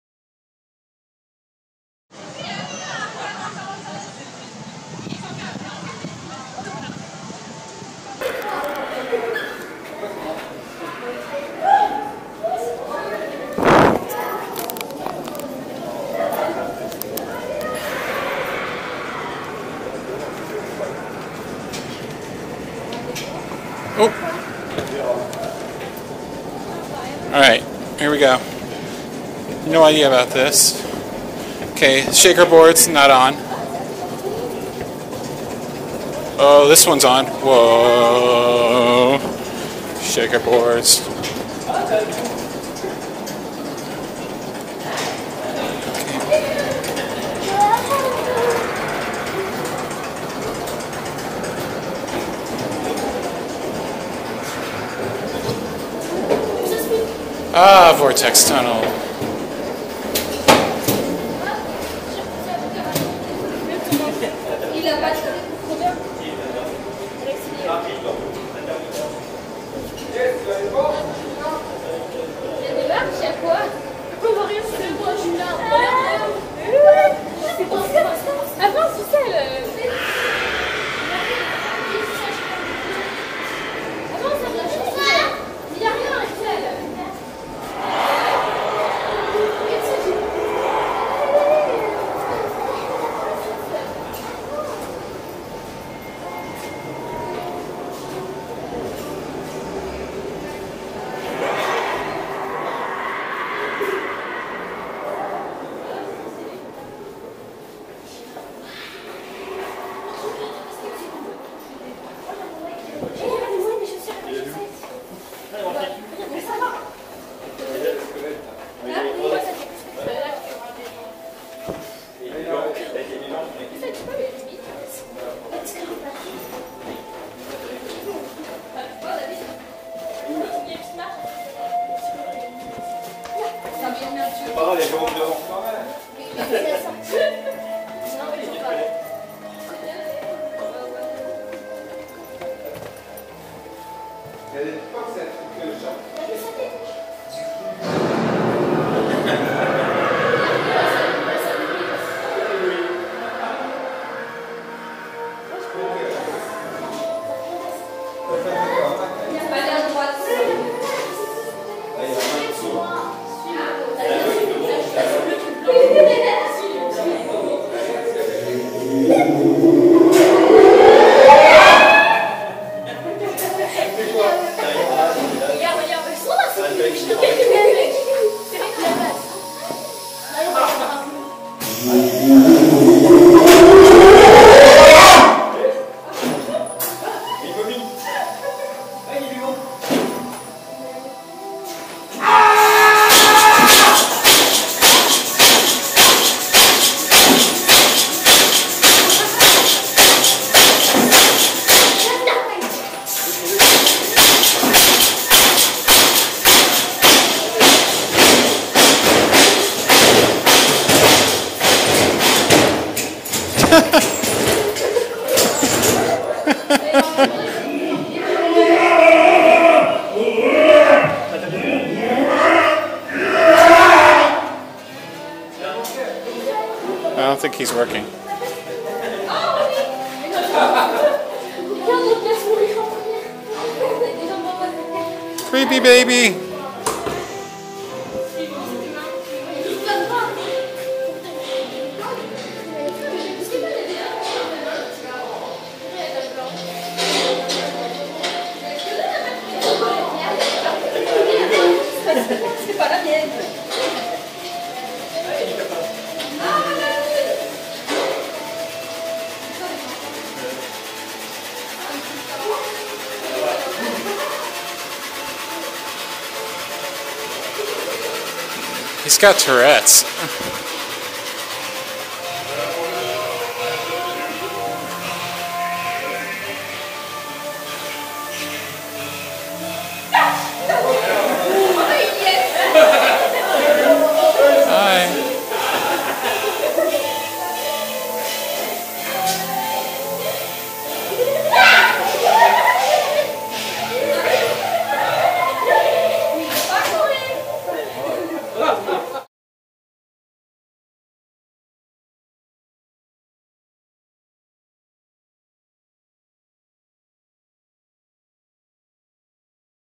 Terror House, Haunted Walkthrough | POV | Walygator Grand Est August 2019
Full walk through of the 2016 all year around Haunted House at France's Walygator Parc.

Feel free to use this footage or any other on/off ride footage in your own videos! My only request is that you provide credit (in video and the description) to this YouTube channel!